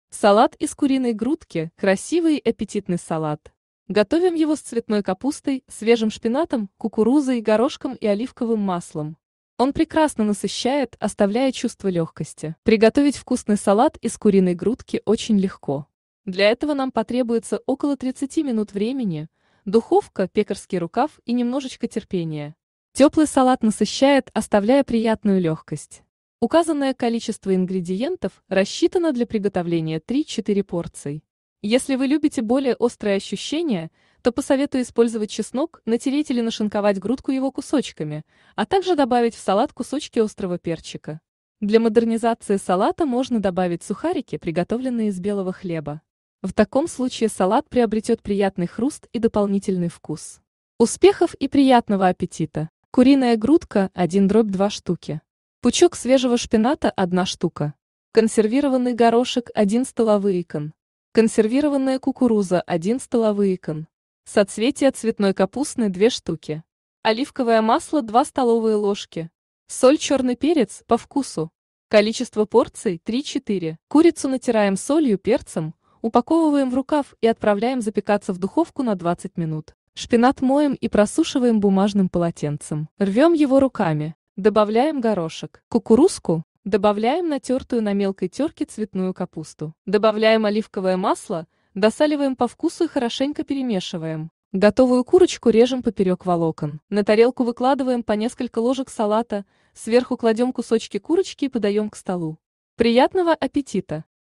Салат из куриной грудки
Салат из куриной грудки - красивый и аппетитный салат. Готовим его с цветной капустой, свежим шпинатом, кукурузой, горошком и оливковым маслом. Он прекрасно насыщает, оставляя чувство легкости.

Ингредиенты:
Куриная грудка — 1/2 Штуки; Пучок свежего шпината — 1 Штука; Консервированный горошек — 1 Стакан; Консервированная кукуруза — 1 Стакан; Соцветие цветной капусны — 2 Штуки; Оливковое масло — 2 Ст. ложки; Соль, черный перец — - По вкусу; Количество порций: 3-4